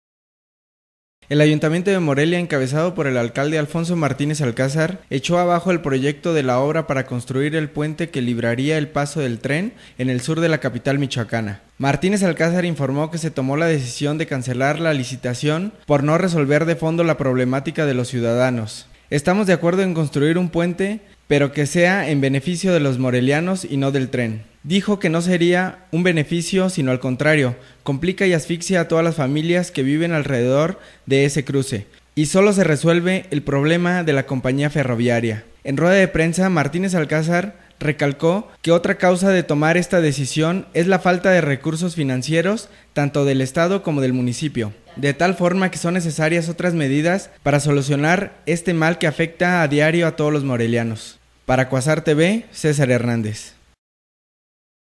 El ayuntamiento de Morelia determinó no ganarle el paso al tren
El ayuntamiento de Morelia, encabezado por el alcalde Alfonso Martínez Alcázar echó abajo el proyecto de la obra para construir el puente que libraría el paso del tren en el sur de la capital michoacana...